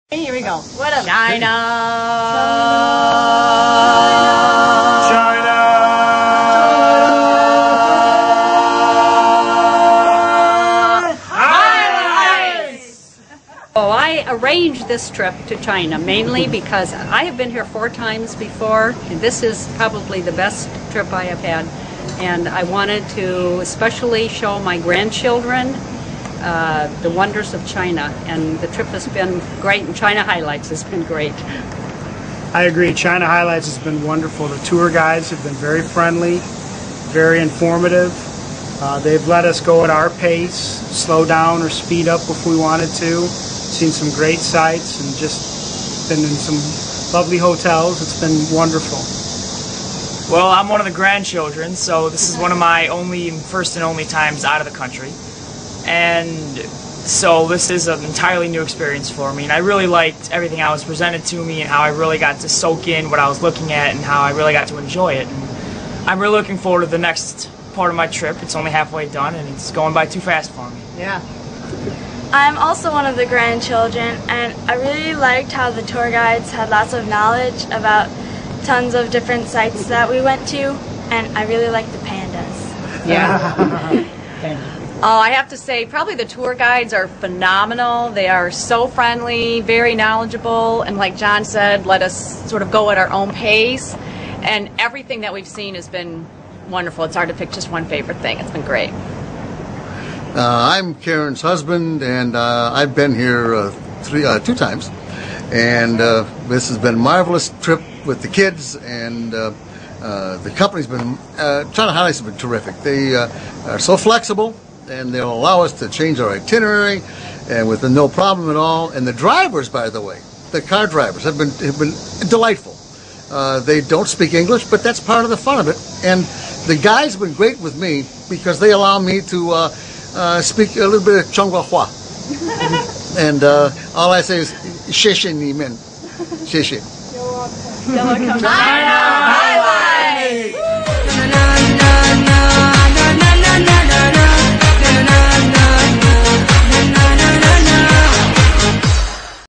Karin's Feedback on 14 Day China Tour - China Highlights
We had several generations in our family group--and there was something of interest to all of us--from the young teens to the grandparents--happening every day.

Tour Itinerary - 14 Days Beijing, Xian, Chengdu, Guilin, Yangshuo, Hong Kong Tour